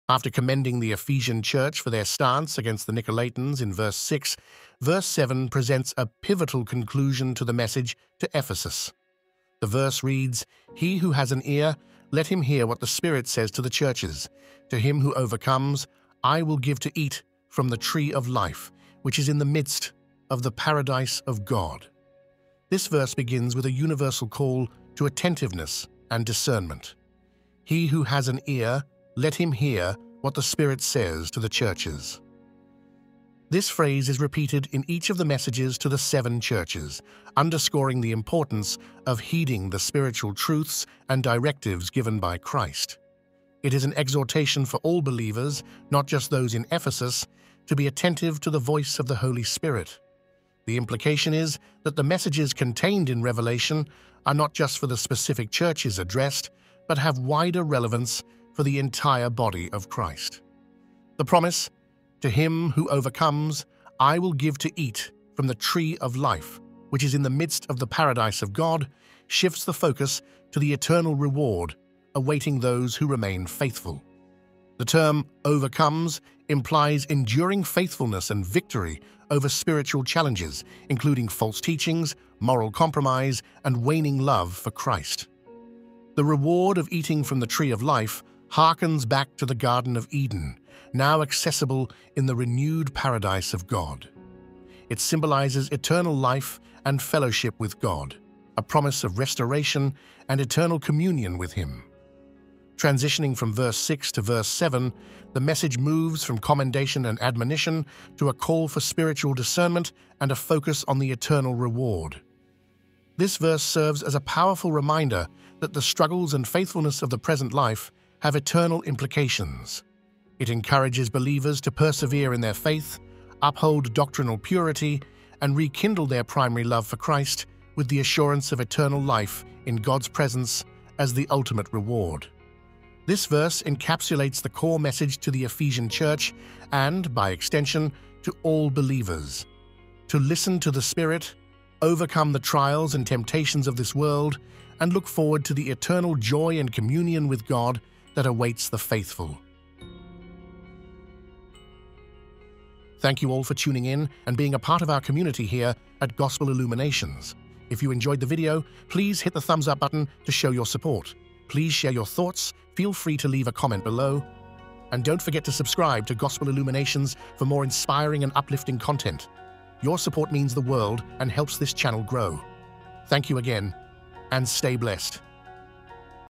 Revelation 2:7 Bible Study: From Eden to Eternity: Understanding the Tree of Life
Revelation 2:7 presents a powerful promise to the believers who overcome: they will be granted to eat from the tree of life, which is in the midst of the Paradise of God. This verse speaks directly to the perseverance and faithfulness required of believers, emphasizing the eternal reward awaiting those who remain steadfast in their faith amidst trials and temptations. The reference to the tree of life harkens back to the Garden of Eden, symbolizing eternal life and fellowship with God, restored through Jesus Christ. It underscores the hope of restoration and eternal communion with the Lord for those who hold fast to their faith. This promise encourages believers to look beyond the temporary struggles of this world, focusing on the eternal inheritance that awaits. It serves as a reminder of the ultimate victory over sin and death, achieved through Christ, and the glorious future that awaits those who are faithful to Him.